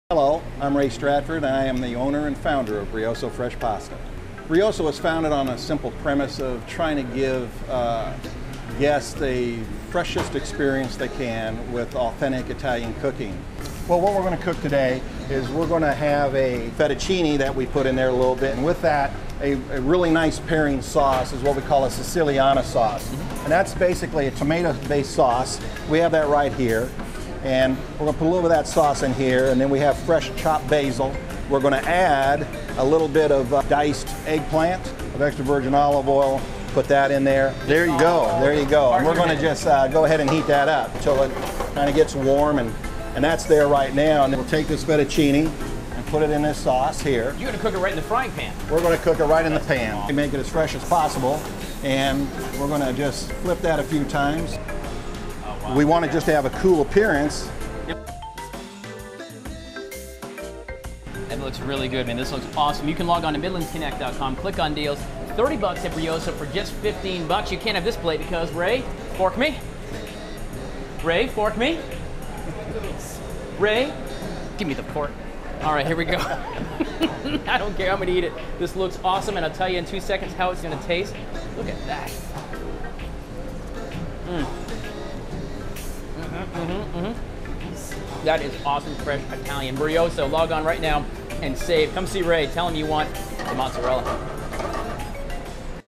WACH Fox Deals: Brioso Fresh Pasta
4/18/12

This WACH Fox Deal is $30 for $15 at Brioso Fresh Pasta in Downtown Columbia!

Two simple words describe the dining experience awaiting you inside..."Brioso Fresh." Using only the best and freshest ingredients, we prepare your order individually, "sous vide" style, preserving the freshness, color, and flavors that define all of our dishes. Enjoy the feeling of stepping into the Italian countryside while enjoying truly fresh, authentic Italian fare at very affordable prices.